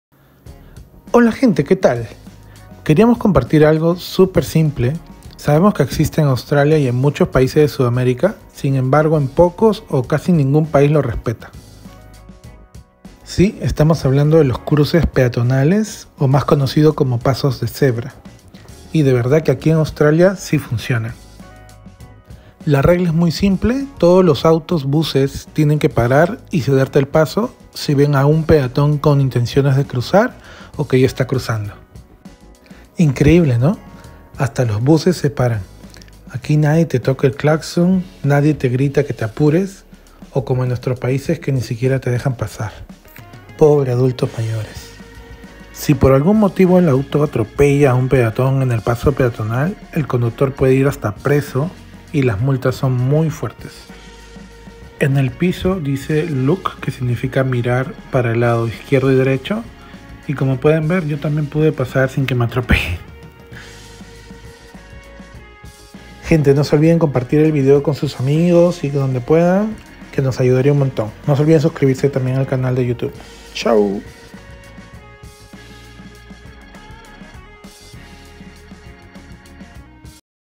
Desafiando el tráfico: Cruces peatonales en Australia
Miren cómo en Australia si respetan e paso peatonal ojalá así fuera en nuestros países así evitamos